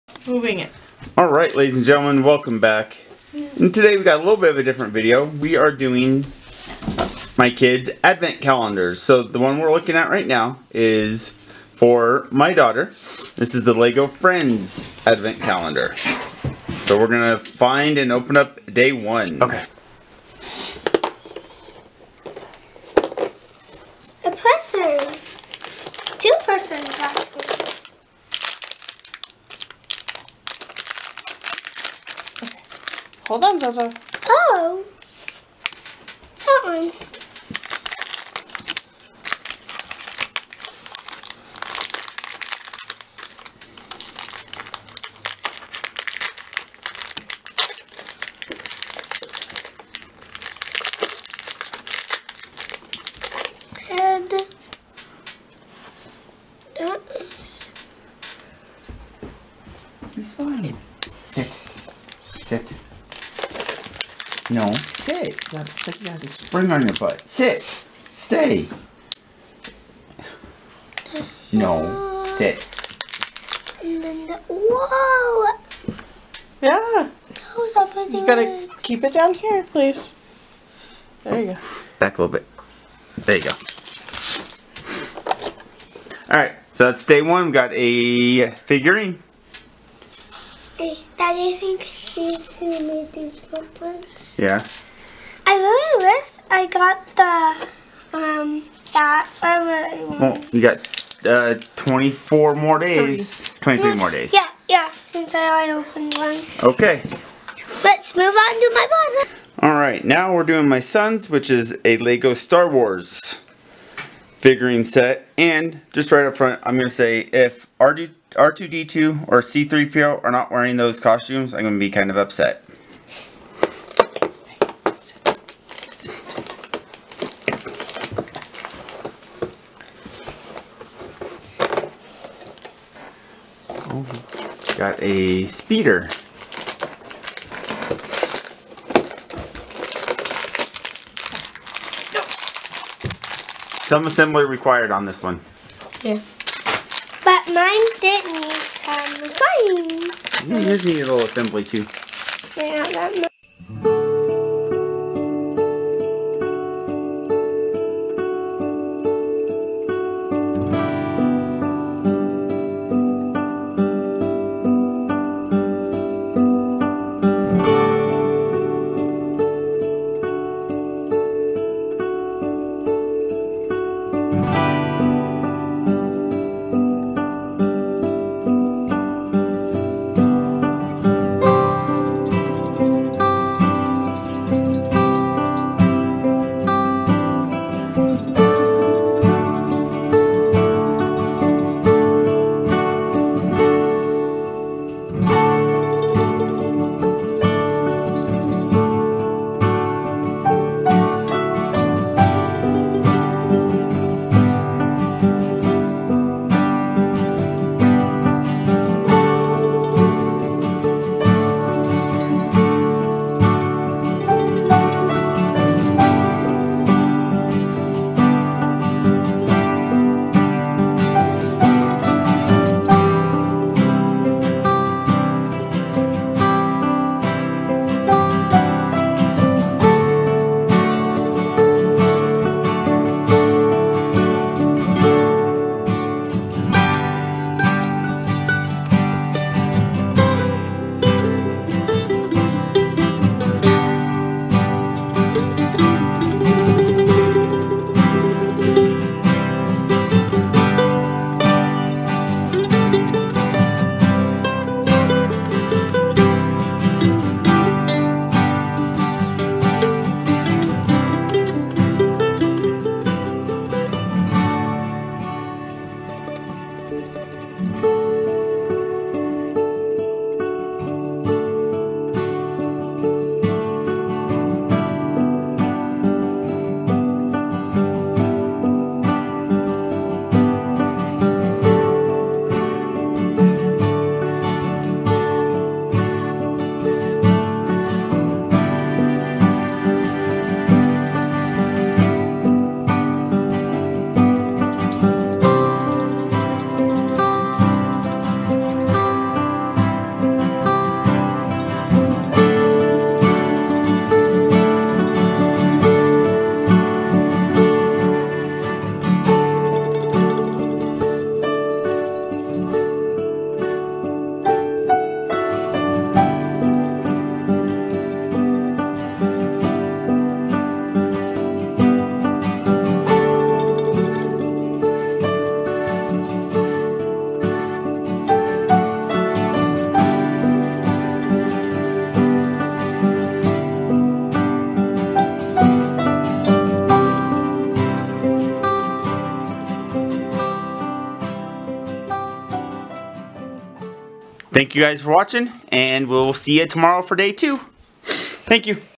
Our First Advent Calendars Dec 1st, 2022
This is the first year my wife and kids talked me into getting advent calendars. We love Legos and these will fit right in with the rest.